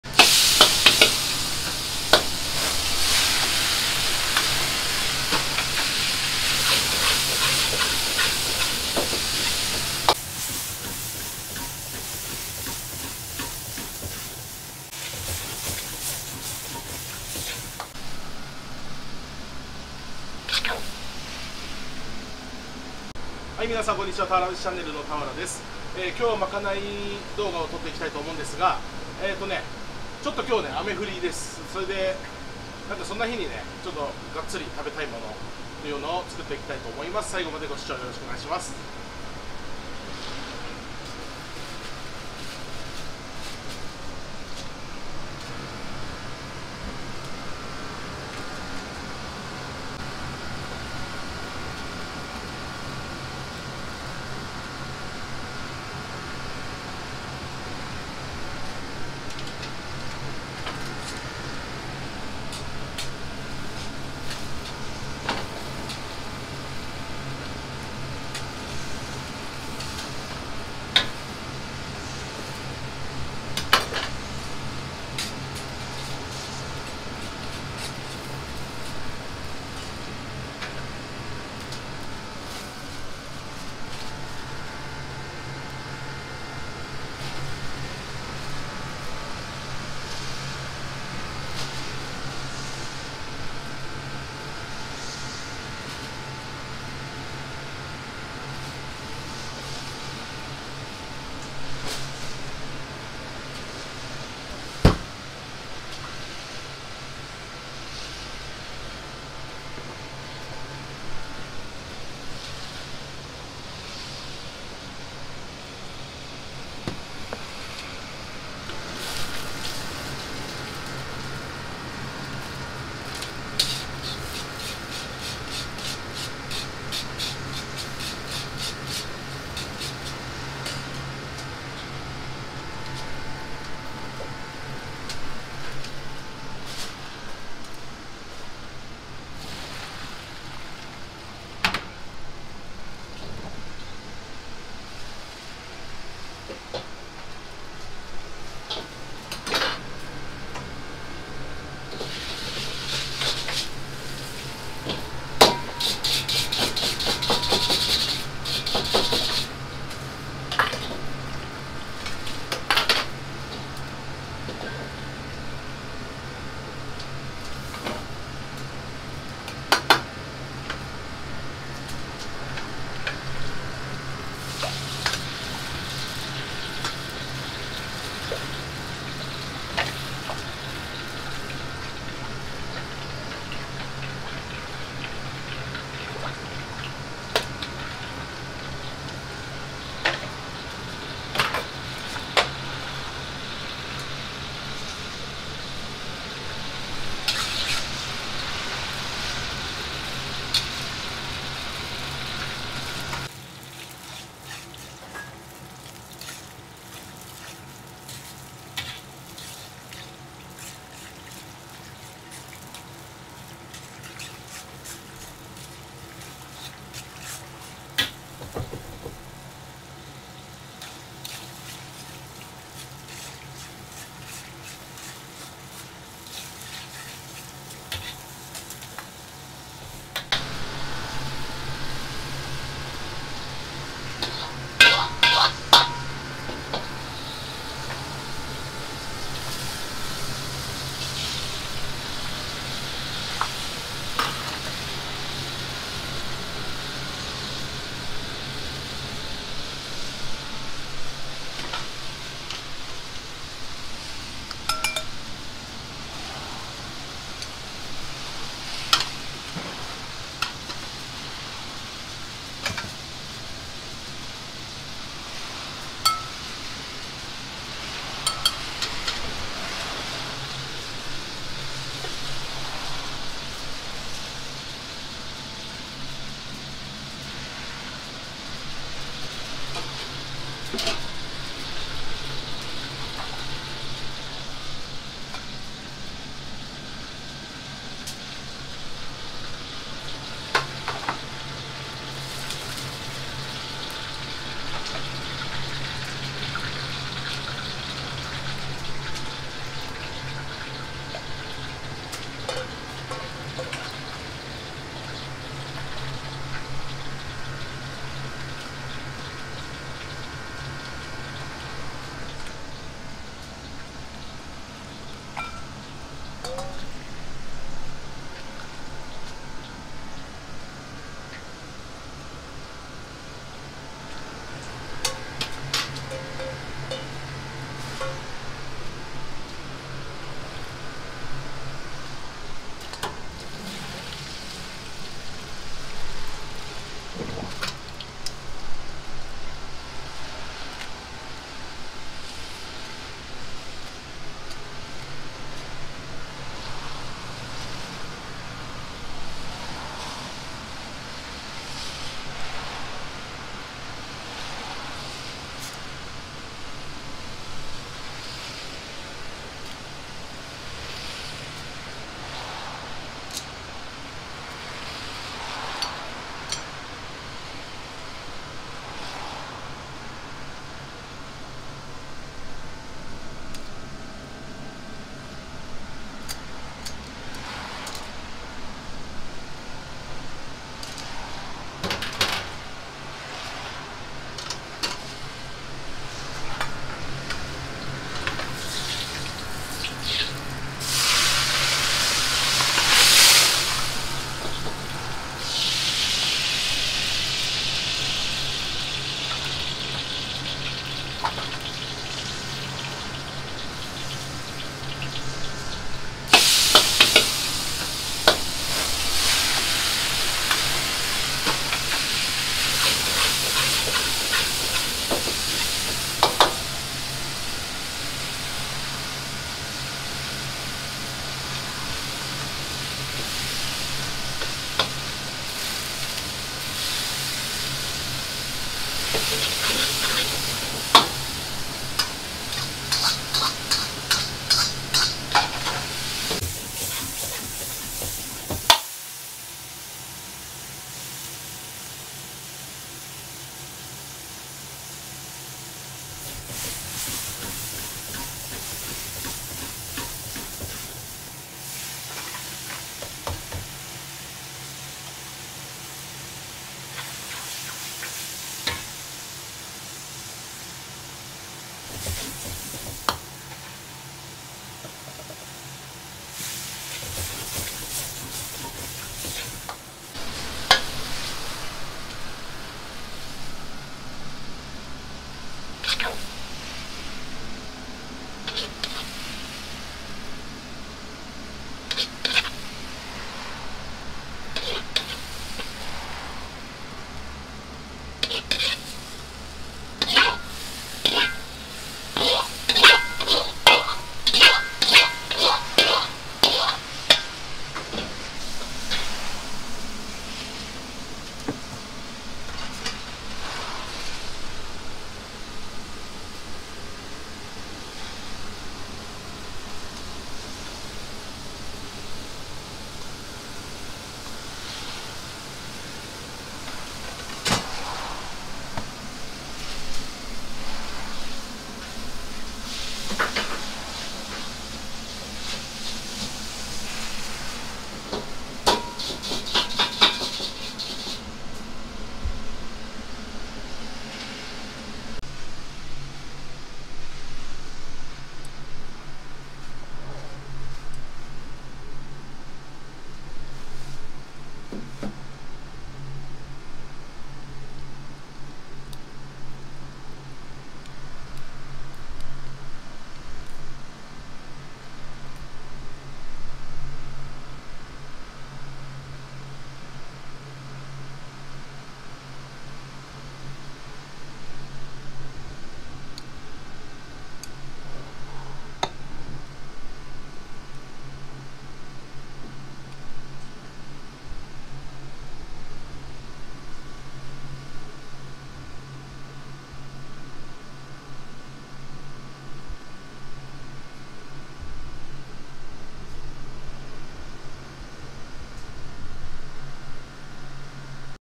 俵の【スタミナ中華そば】ワンバン用ライスと一緒に♪
下は生姜風味のシンプル中華そばスープ。
上には肉ニラもやしのスタミナ炒め！
【俵のスタミナ中華そば】の完成です^ ^
ワンバン用ライスと一緒にまかないです♪